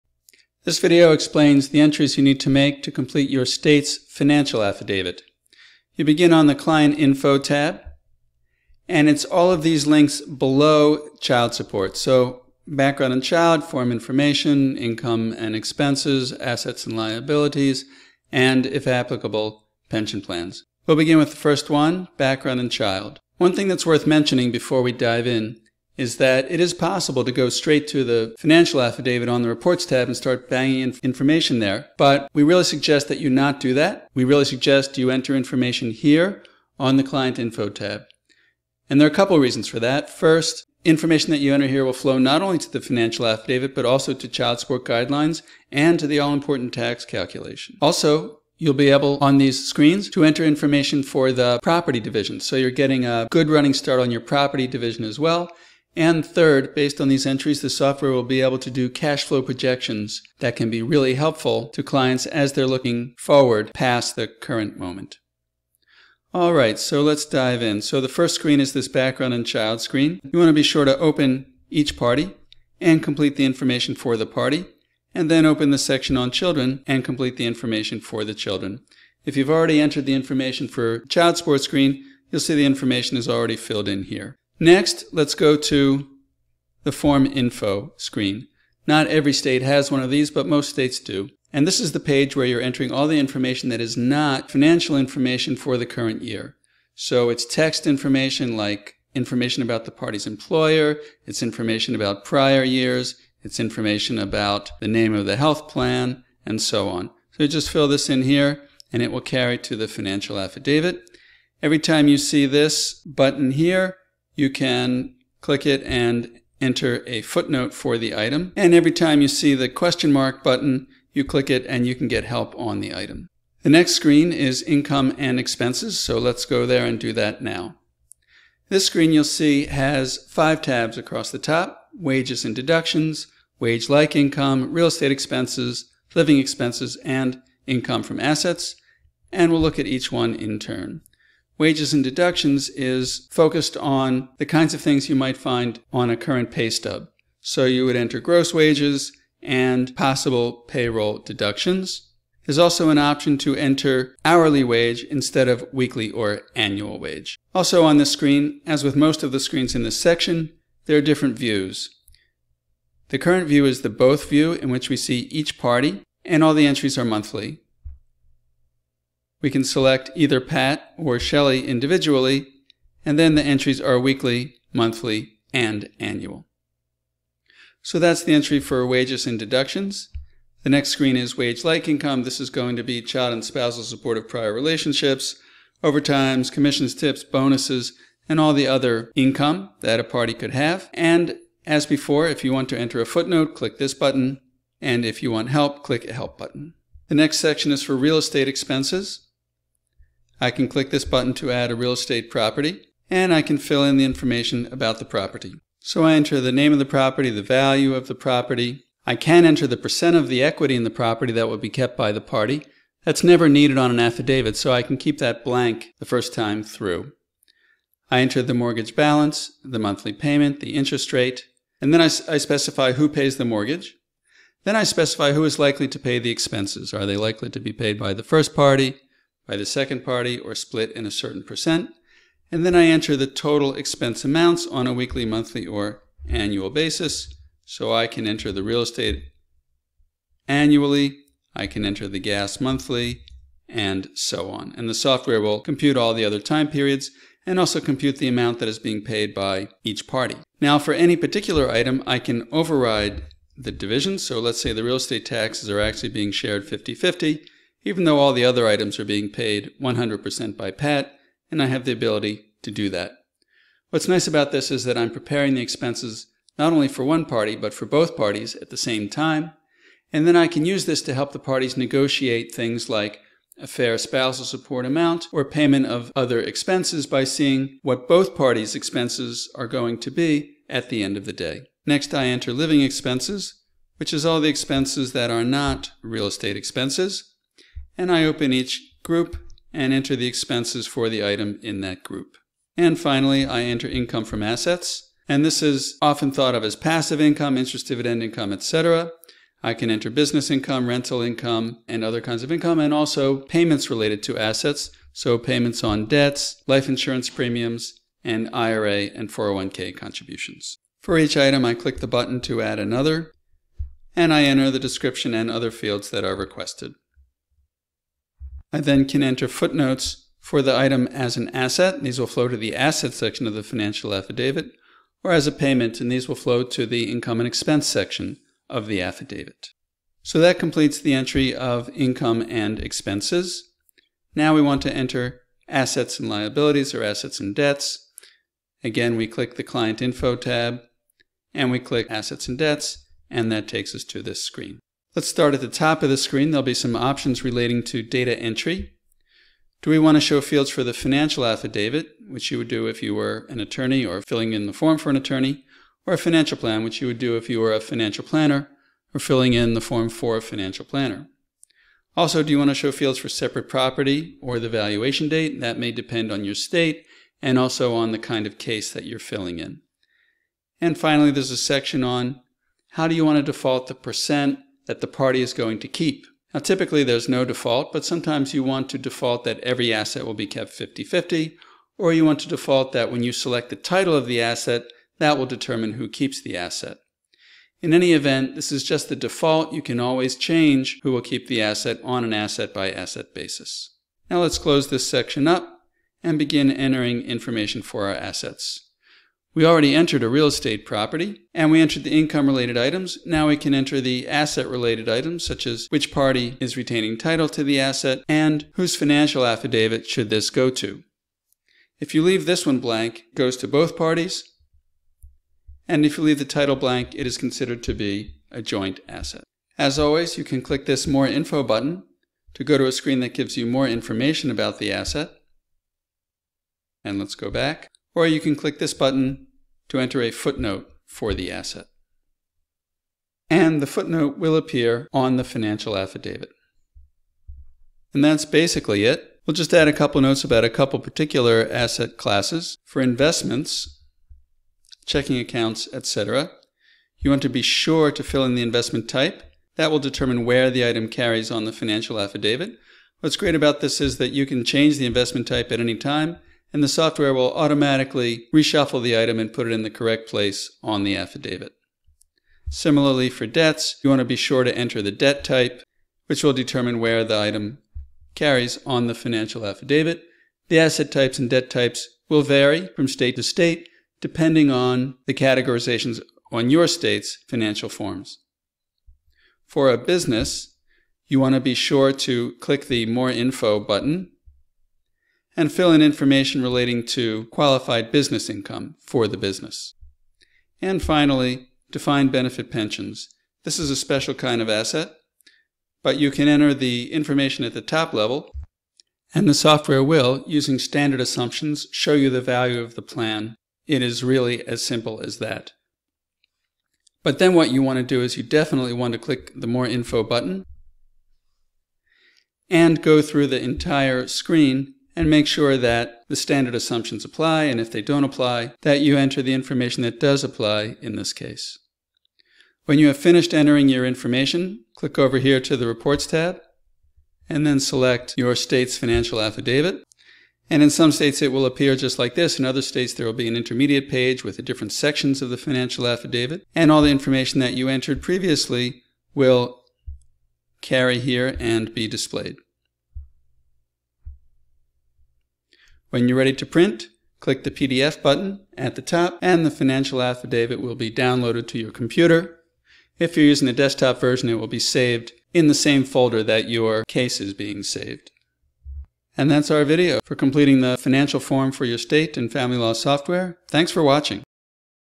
How to Enter Information for Financial Statements in Family Law Software
Family Law Software makes it straightforward to enter information for clients financial statements. Without duplicate entry, that same information is used to do accurate budgeting, taxes, and cash flow projections. You can also enter footnotes, which then can be located on the form where you want them. This video explains how to enter the client's information into Family Law Software. This is a great introduction for paralegals, and is guaranteed to speed the learning process.